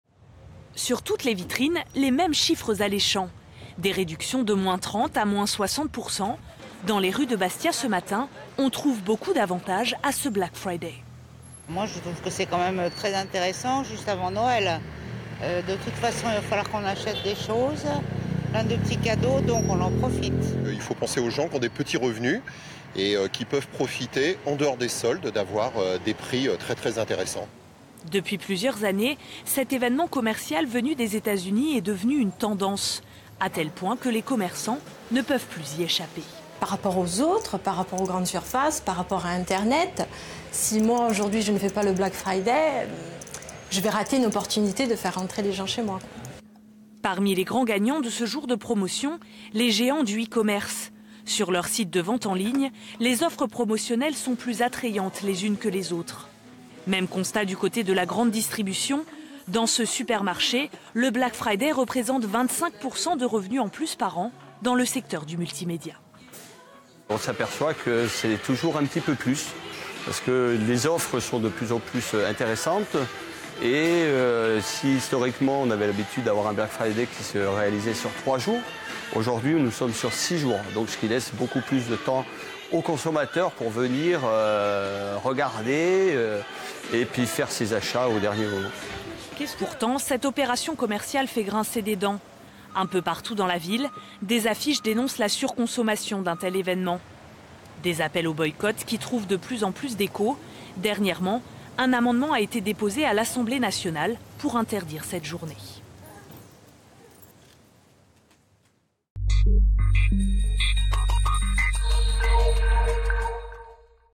Corse : le Black Friday entre offres promotionnelles alléchantes et appels au boycott
C'est une date qui en quelques années, s'est installée dans le calendrier commercial des Français. Le Black Friday, une grande journée de promotions à un mois de Noël, très attendue par les consommateurs, mais qui font aussi grincer des dents. Illustration dans les rues de Bastia.

Intervenants - Emmanuelle Luciani, Responsable boutique de prêt-à-porter ; Franck Benet, Directeur Casino Furiani.
Equipe - Camille Wormser ; Pierrick Nannini ; Wendy Martini.